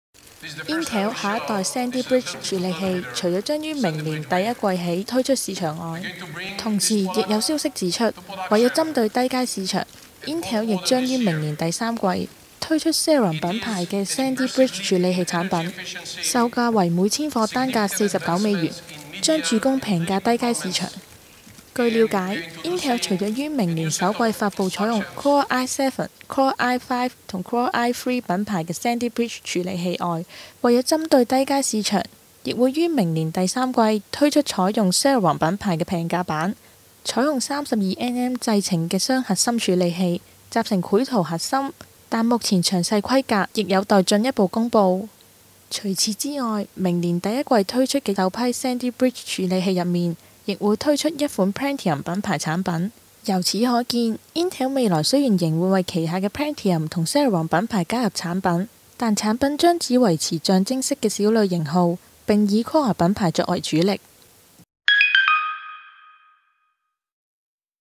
2011年Q3推出Celeron Sandy Bridge每千顆單價49美元
Intel 下一代 Sandy Bridge 處理器除了將於 2011 年第一季起推出市場外，來自台灣 PC 業者指出有消息指出，為針對低階市場， Intel 亦將於 2011 年第三季推出 Celeron 品牌的 Sandy Bridge 處理器產品，售價為每千顆單價 49 美元，將主攻平價低階市場。